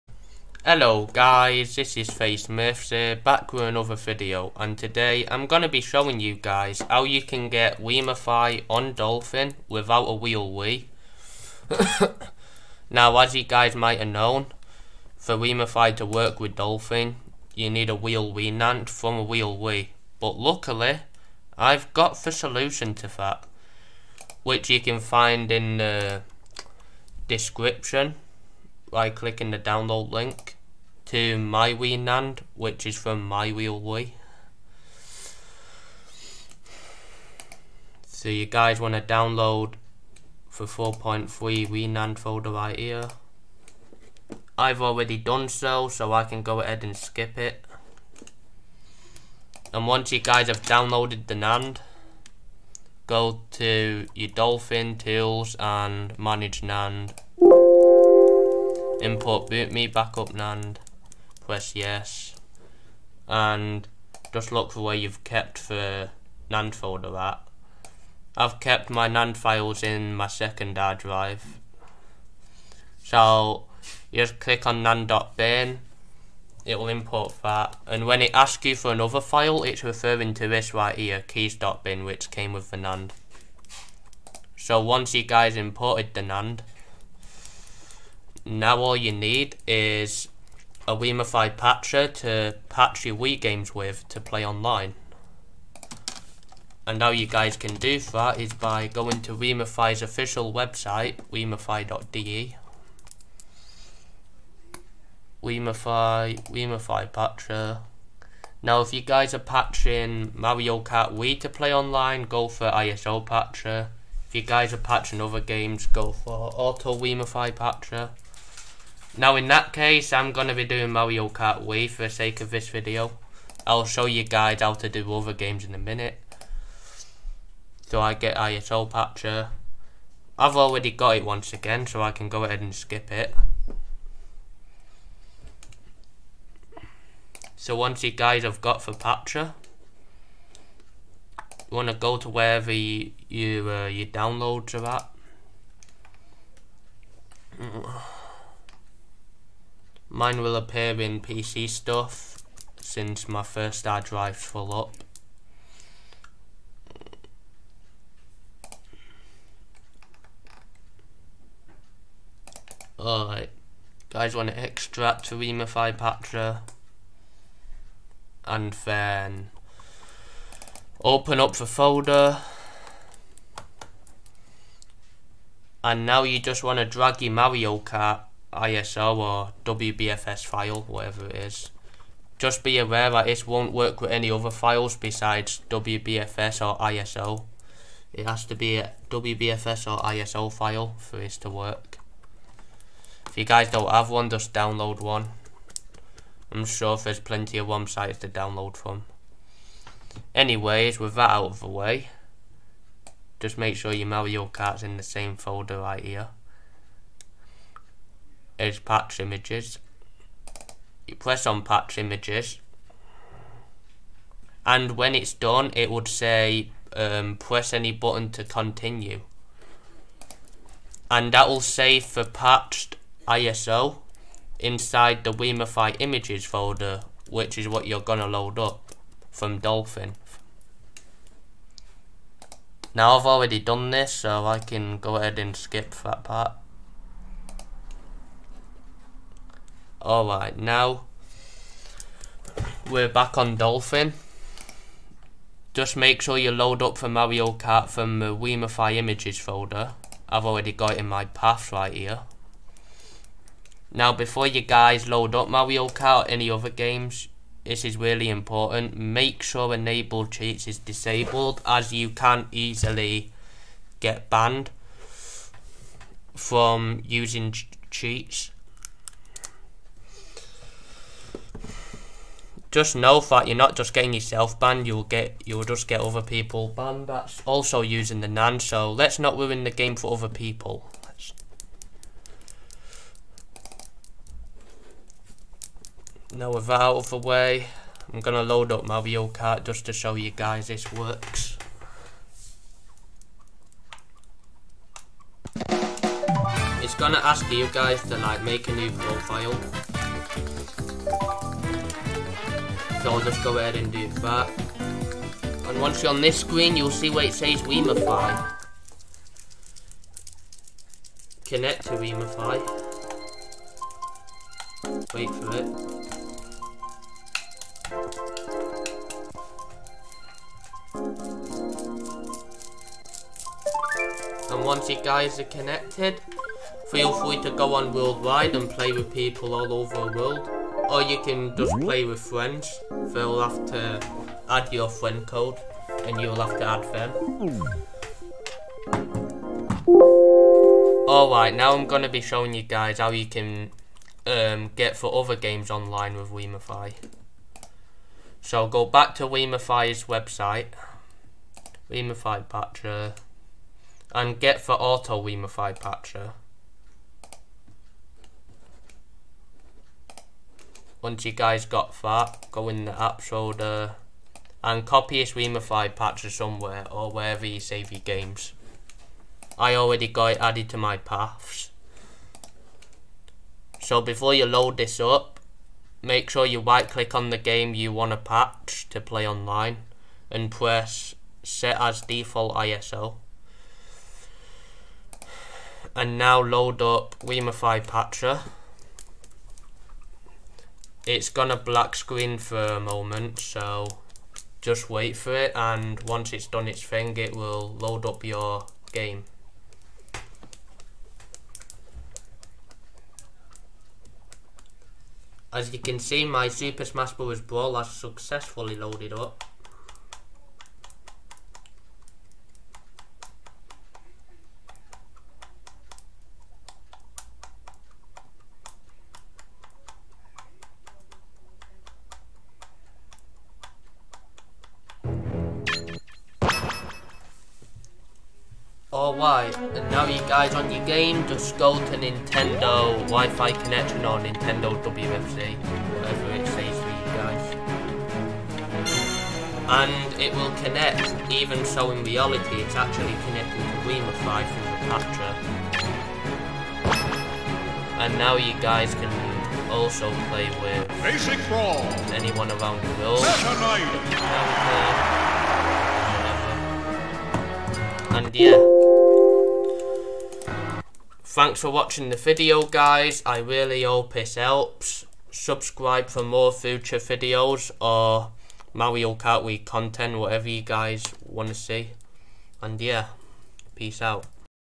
How to get Wiimmfi on dolphin without a real wii (outdated)
If you get error code 23911 it means you have to wait 24 hours if it says the same thing wait another 24 hours till it lets you on